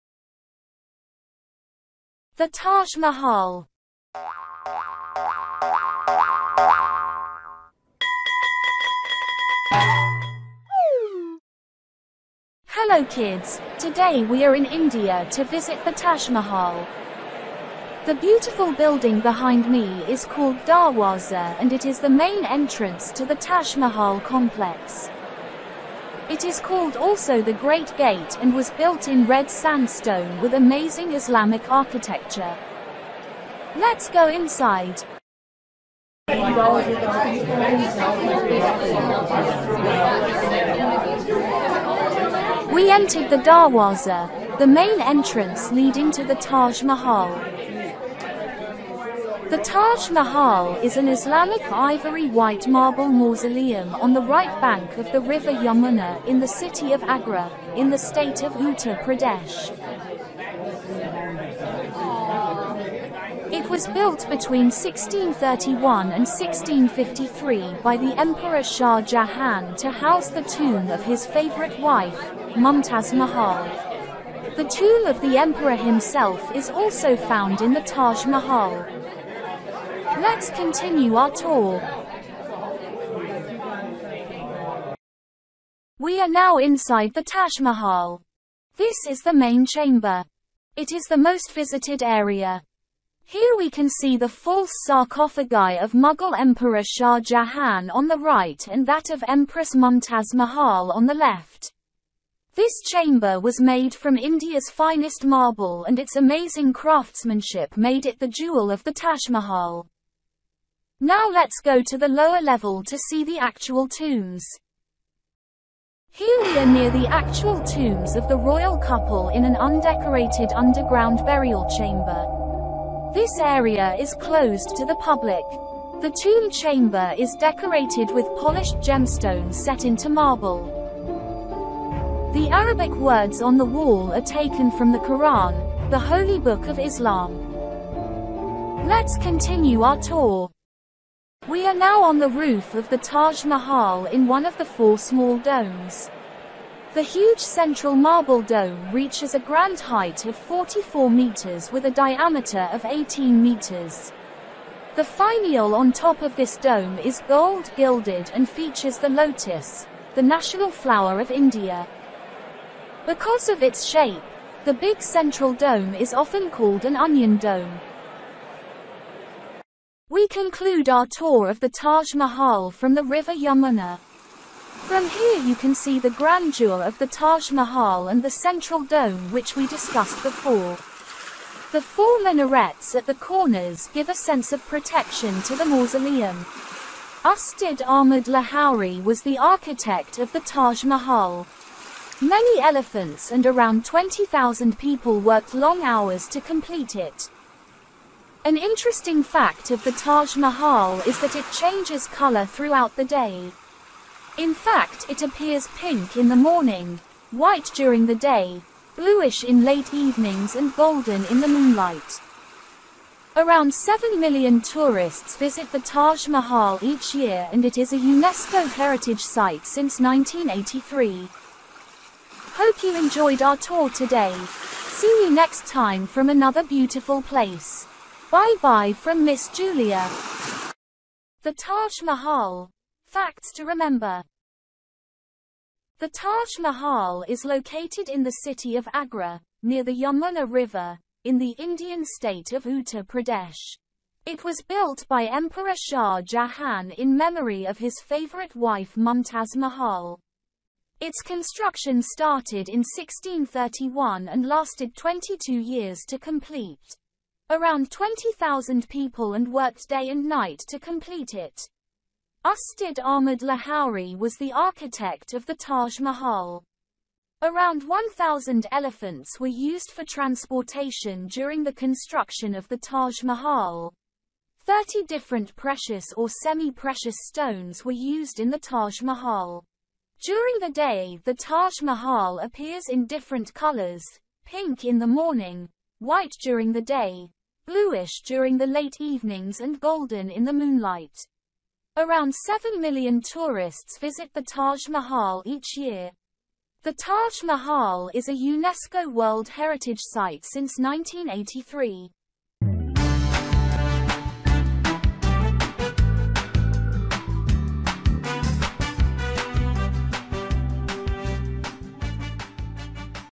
Taj Mahal - Fun Learning Adventure For Kids
Hello kids! Here is another educational tour around one of the most important landmarks in the world: the Taj Mahal in India!

Although this video is mainly targeted at kids, everyone can learn from this historical educational tour.
Join Miss Julia on her cultural voyages around the world, where she visits important places in various countries. In this video, she will show you around one of the most important landmarks of India and Islamic civilization—the Taj Mahal has a rich history indeed!
I hope you enjoy these videos. Remember kids…Learn and have fun with Miss Julia!

Please watch the complete US history quiz... "US History Mega Quiz - 50 Questions quiz quizgame quiztime "